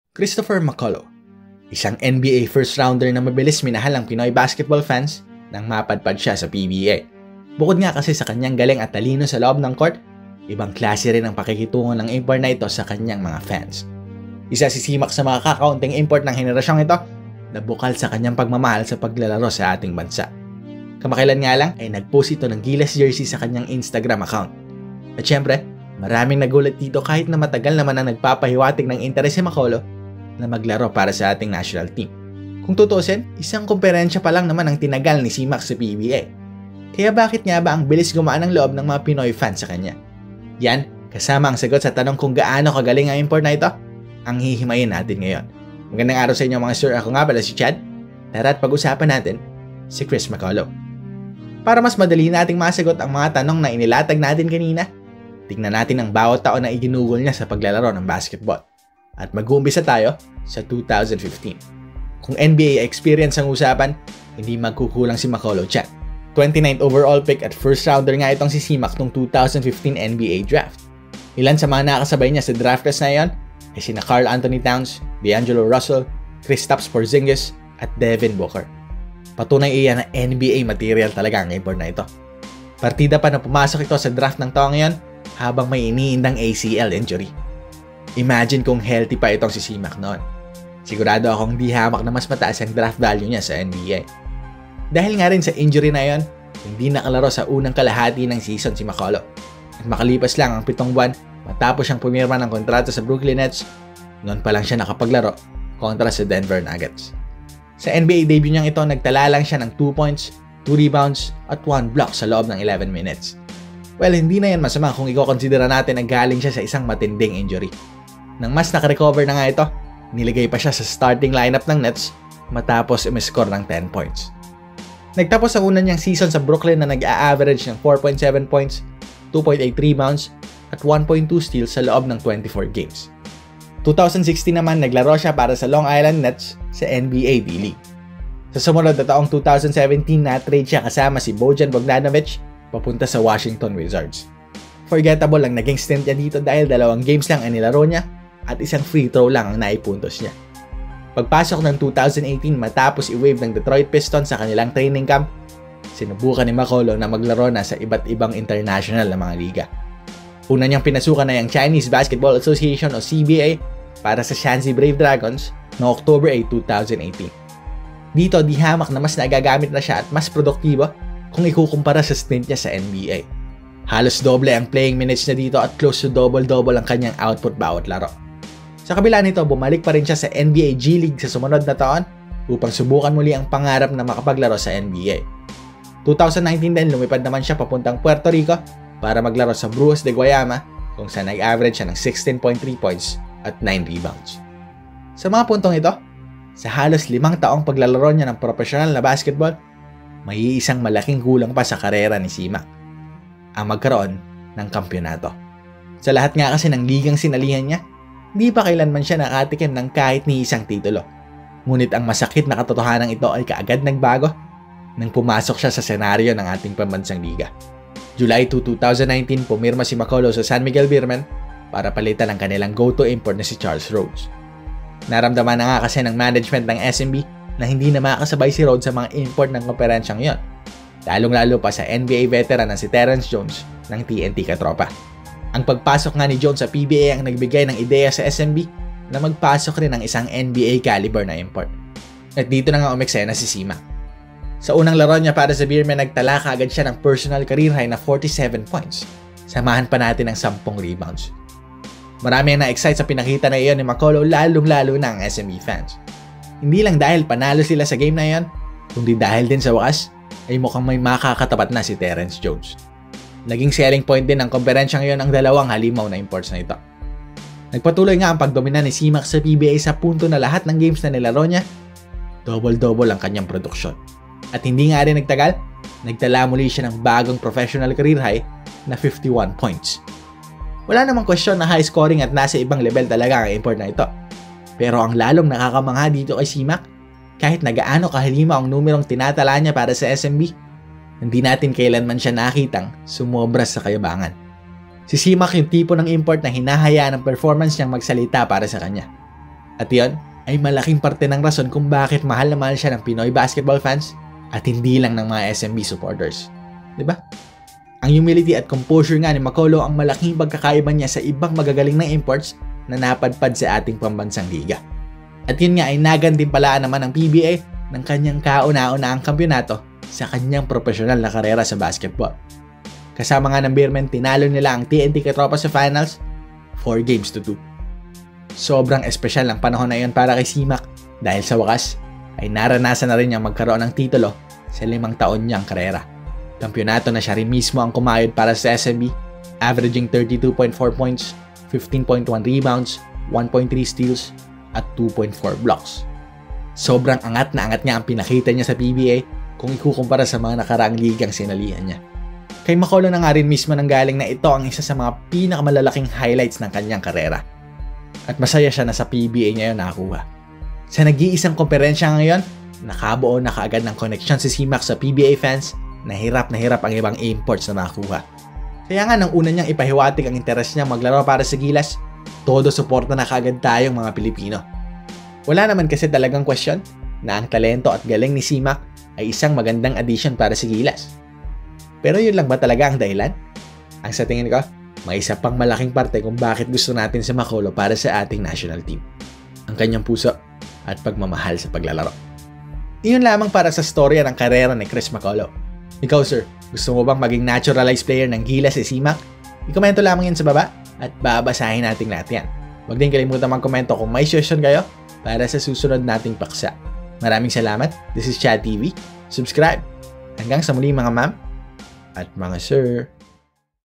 THE CHRIS MCCULLOUGH STORY | Paano at Bakit Nga Ba Sya Kaagad na Minahal ng Pinoy Basketball Fans?
Kilalanin ang isa sa mga napamahal na PBA imports sa ating bansa. Bakit nga ba mabilis nating tinanggap ang pagdating ni Chris McCullough at sya na nga ba ang pinakahihintay nating pupuno sa kakulangan ng ating national team? Tara't pag-usapan natin.